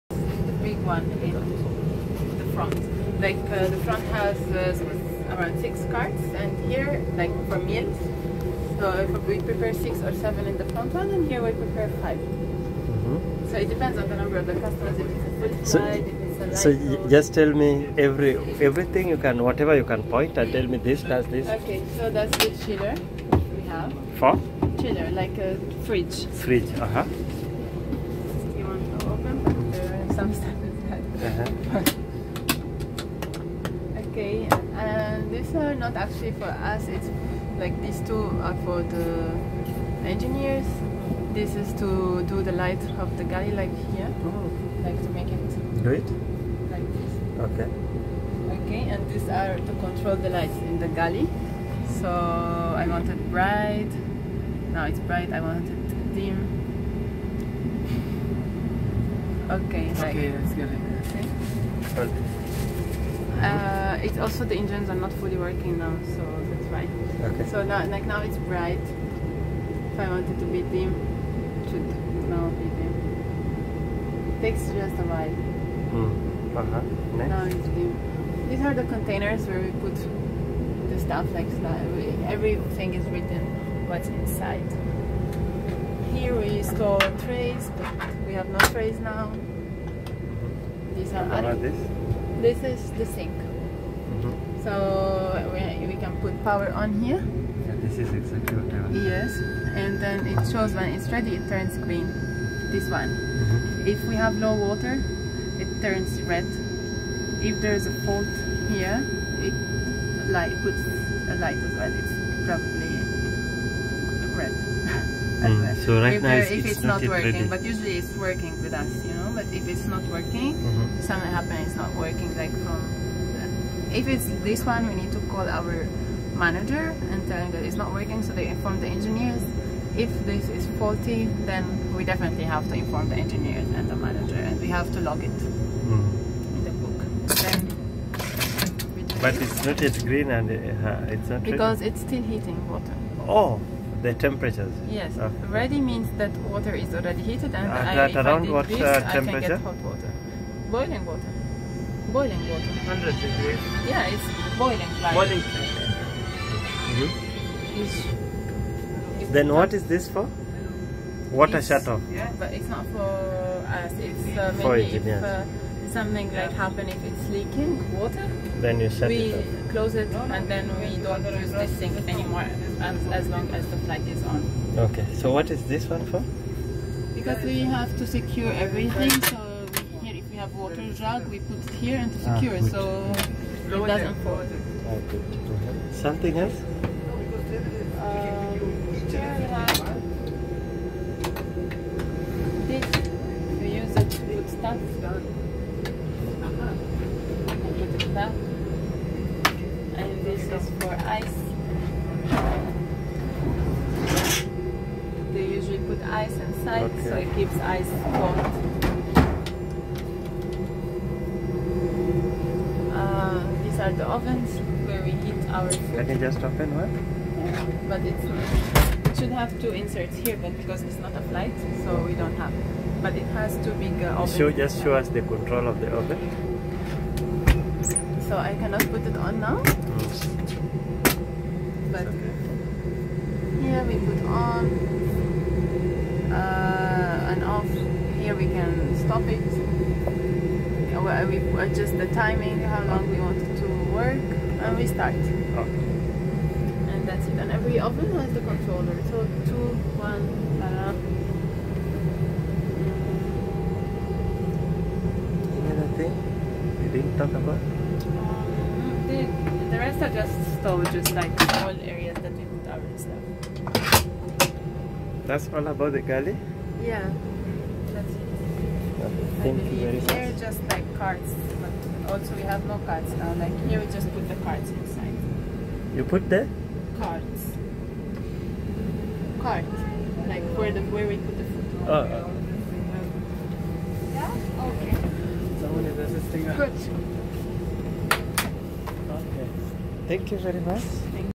Emirates Airbus A380 Galley Unit Operation explained by the fabulous Emirates crew at Dubai Airshow.
Emirates crew superbly explaining the working of the Galley unit during Dubai Airshow 2019.